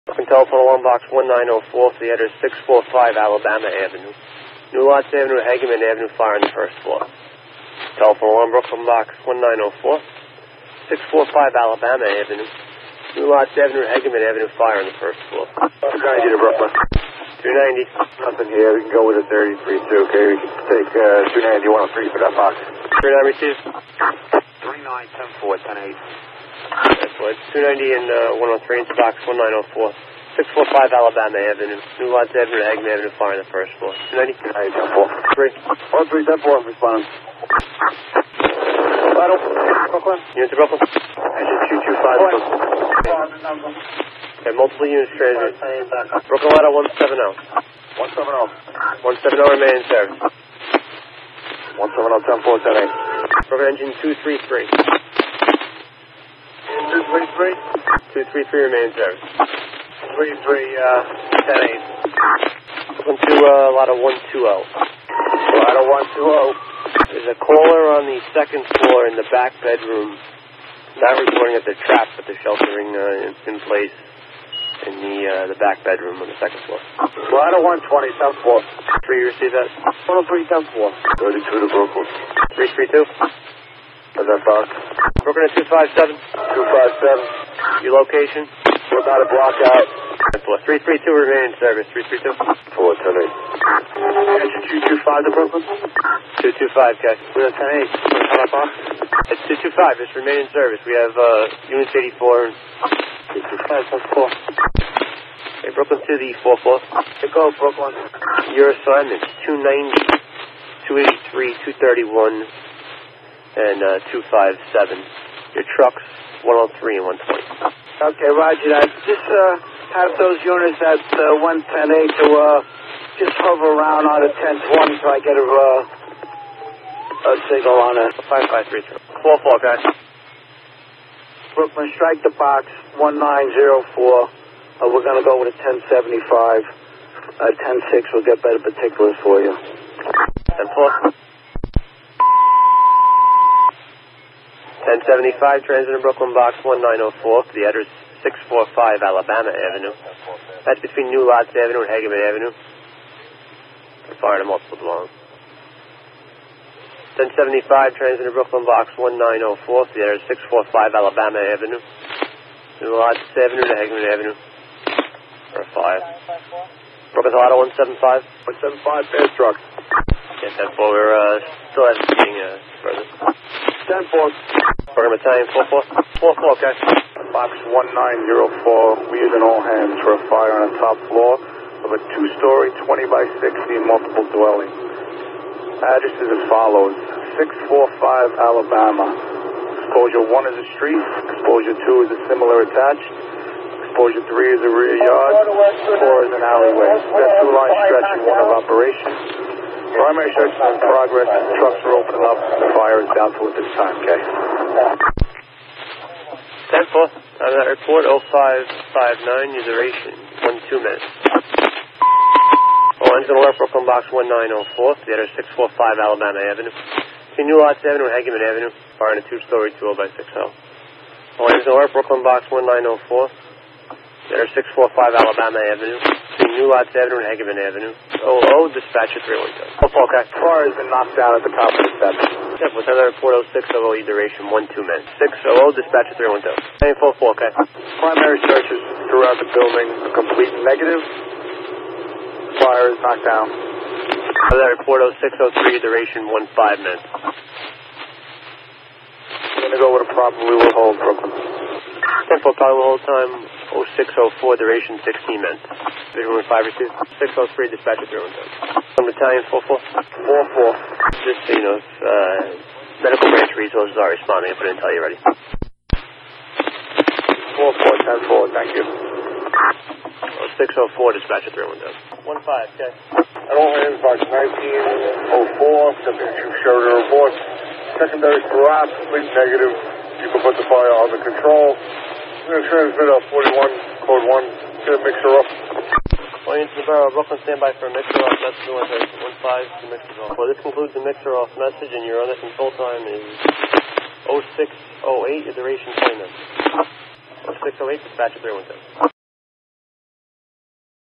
NYCFireAudio - Brooklyn All Hands Box 1904 - Fire In A Private Dwelling - 12/31/17
AUDIO PROVIDED BY BROADCASTIFY

Fire Location: 645 Alabama Ave

B-44 “Strike the Box. Go with the 10-75”

Fire on the top fl of a 2 story MD

E-290,283,231,257
L-103,120,175(Fast)
B-44,58
R-2
Sq-252
D-15
Rac-2

B-44 All Hands

*Was a Box out prior to this incident*